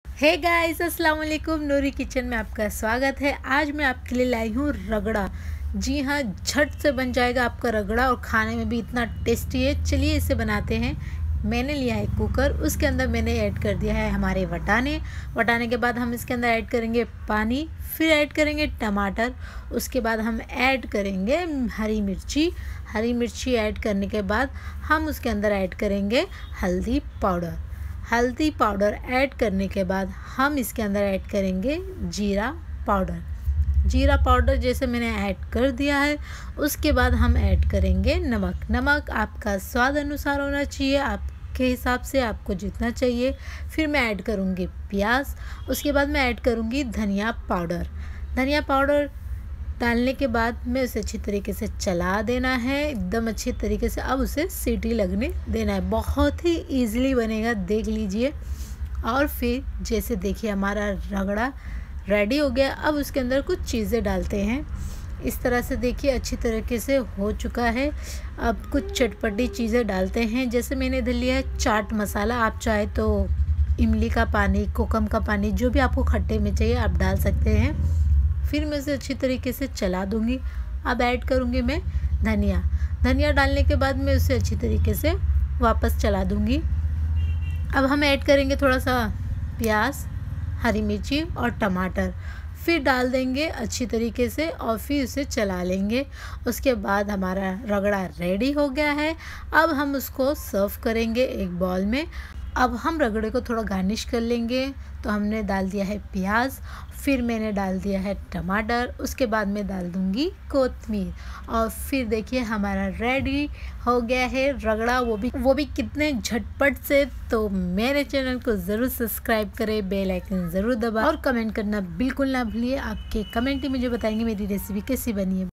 Jhatpat ragdaa banaye wo bhi 10 min me/jaldi se banaye ragdaa/how to make ragdaa ❤️December 26, 2022
Jaldi se banaye Jhatpat ragdaa
Ragdaa banaye wo bhi 10 min me
How to make ragda ❤️🥰👍

Recipe:
1.onions
2.green chili
3.tomato
4.turmeric powder
5.coriander powder
6.cumin powder
7.salt
8.chaat masala
9.coriander